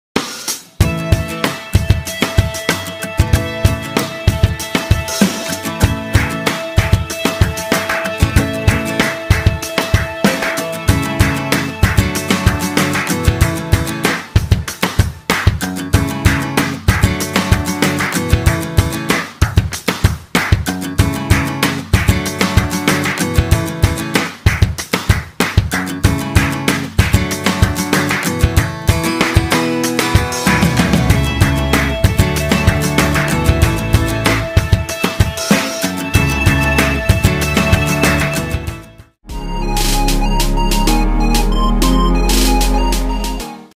Sessão Azul Em São José Dos Campos-SP
8/12/2018 foi realizado no shopping Vale Sul no cinema Kinoplex e Divertion teve o prazer de tirar as fotos